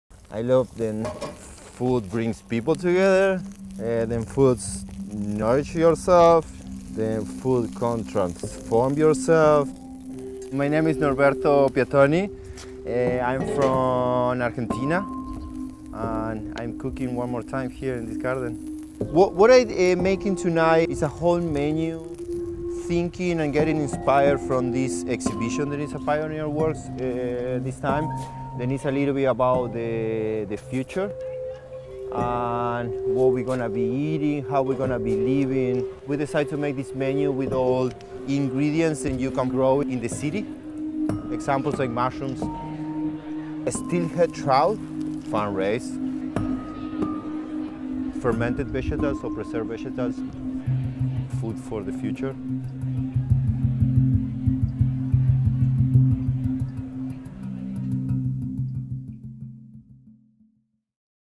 Norberto Piattoni | Supper Club
Norberto Piattoni, the first chef of the Supper Club series, returns to Pioneer Works in celebration of the exhibition Atelier Van Lieshout: The CryptoFuturist and The New Tribal Labyrinth. The menu—created in response to the exhibition’s thematic focus on sustainability—highlights alternative modes of food production by using ingredients sourced from local farms and synthetic environments, and prepared through Piattoni’s renowned techniques of wood fire and fermentation. Have a first look at the exhibition with our guest of honor, artist Joep Van Lieshout. Pioneer Works Supper Club is a dinner series created to celebrate local chefs and serve as a community gathering around an original dining experience.

Norberto Piattoni was born in Federación, Argentina—where he trained with renowned Argentine chef Francis Mallmann. Norberto worked as head chef for four seasons at Mallman’s much-lauded Restaurant Garzon in a romantic Uruguayan ghost town. In 2014 he moved to San Francisco to sous-chef at Bar Tartine, where he became passionate about natural fermentation and an eastern European style of preserving and cooking. In 2017, he opened acclaimed neighborhood restaurant, Mettā, in Fort Greene, Brooklyn, serving a locally sourced seasonal menu cooked entirely with fire. Norberto’s approach to cooking is centered around sustainability. Piattoni is dedicated to finding creative solutions to utilize all parts of the plant/animal when cooking, thus eliminating waste and repurposing ingredients whenever possible. Piattoni procures local produce at its peak and employs fermentation techniques of brining and pickling to make ingredients last through the leaner months.